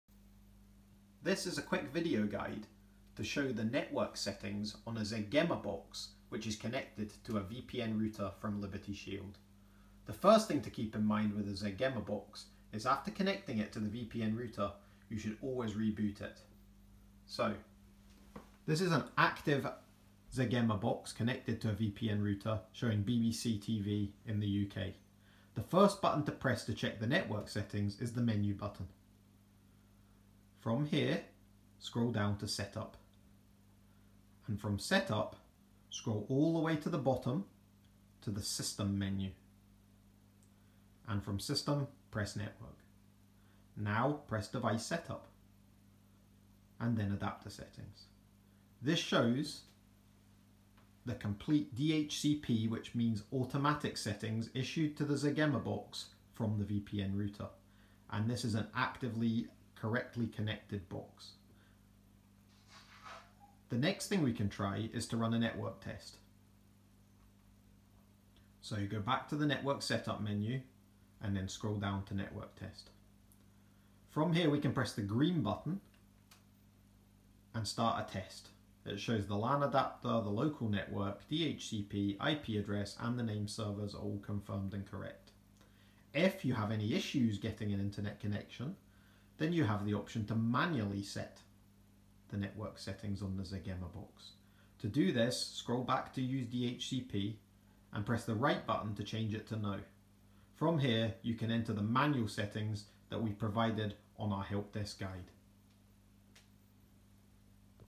Checking Zgemma Connection to VPN Router
Quick video guide on how to check that your Zgemma is connected correctly the VPN Router.

This Zgemma is connected correctly to a Liberty Shield UK VPN Router to bypass ISP blocks.